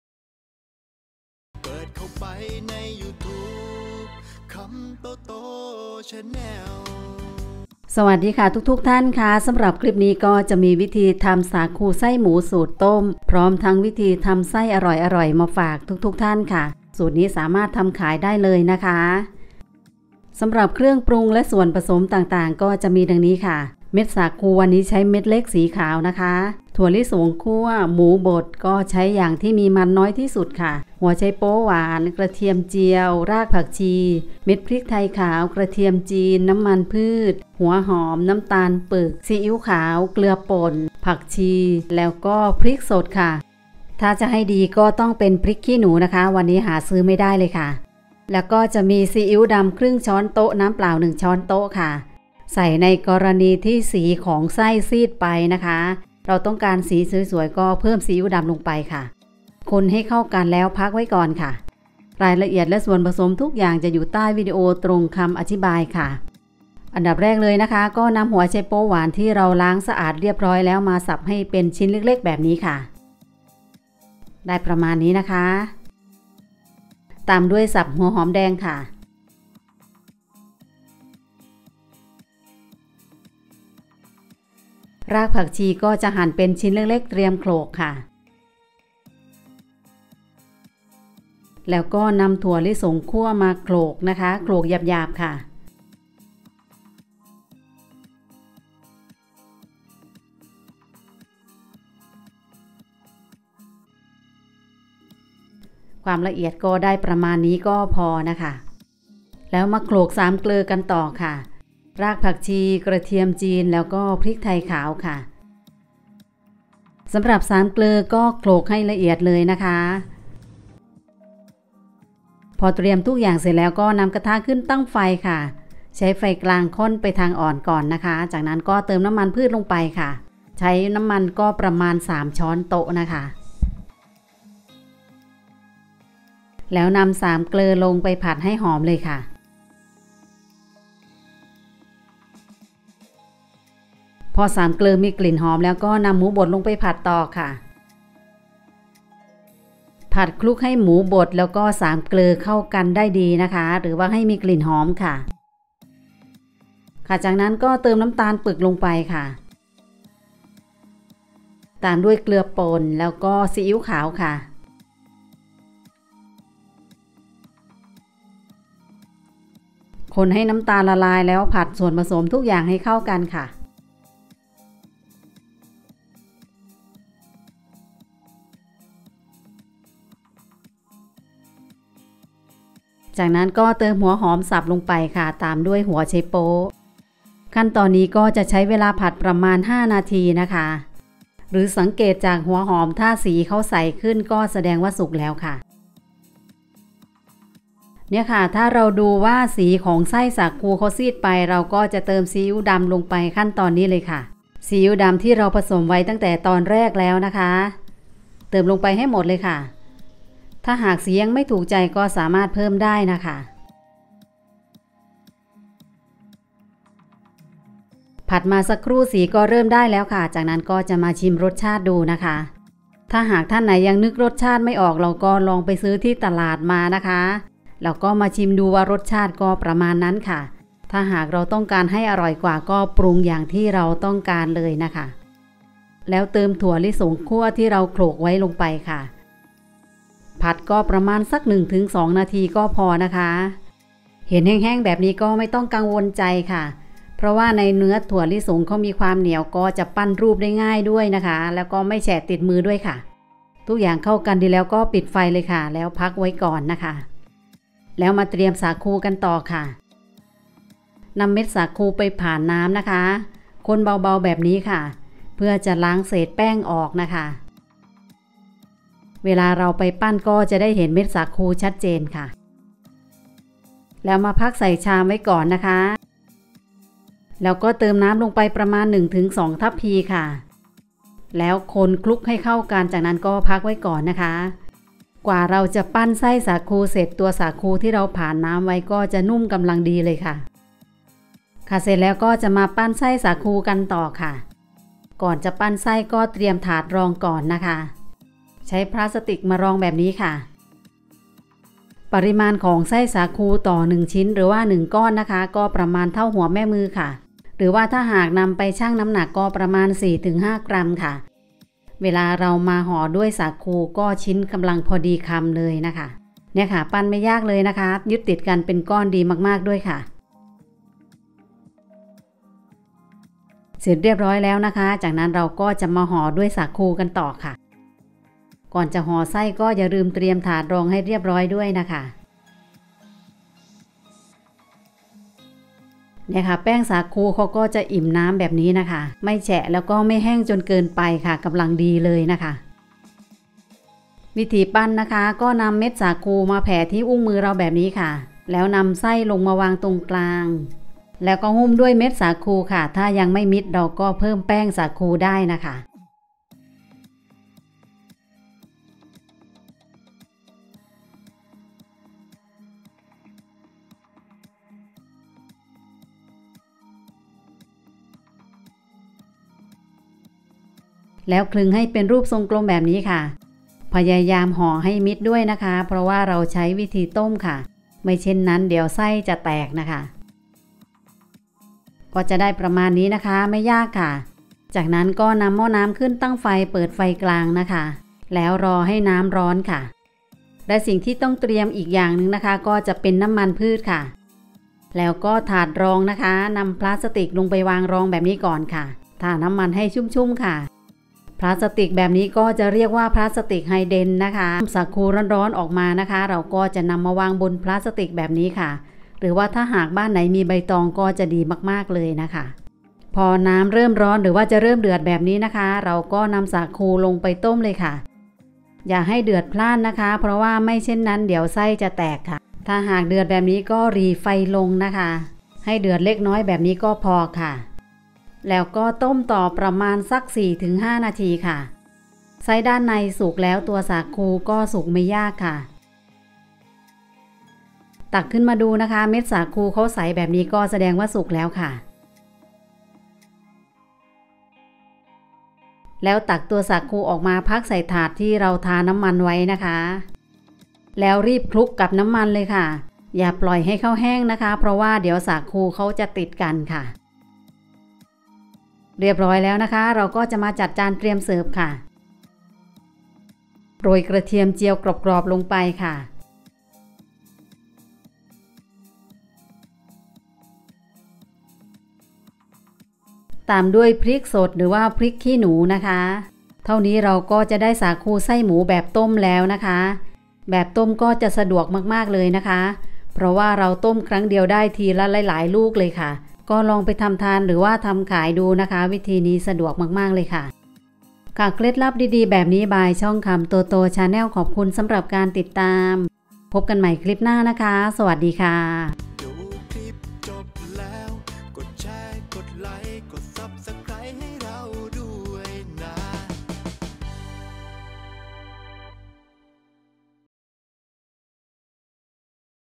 สาคูไส้หมูสูตรต้ม สาคูไส้หมู พร้อมสูตรทำไส้สาคูอร่อย บอกละเอียด สร้างอาชีพ
เครื่องปรุงและส่วนผสมต่างๆของสาคูไส้หมู
สาคูสีขาวเม็ดเล็ก            250          กรัม
หมูบด                              200          กรัม
ถั่วลิสงคั่ว                        120          กรัม
ไชโป๊ยหวาน                   150         กรัม
หัวหอมแดง                     50           กรัม
เม็ดพริกไทยขาว             1+1/2     ช้อนชา
กระเทียมจีน                     5             กลีบ
น้ำตาลปึก                       2+1/2       ช้อนโต๊ะ
น้ำมันพืช                          1/2         ถ้วยตวง (สำหรับผัดและคลุกสาคู)
ผักชี
หากท่านใด มีข้องสงสัยหรือไม่เข้าใจอะไรฝากคอมเมนต์ไว้ใต้ วีดีโอได้เลยค่ะ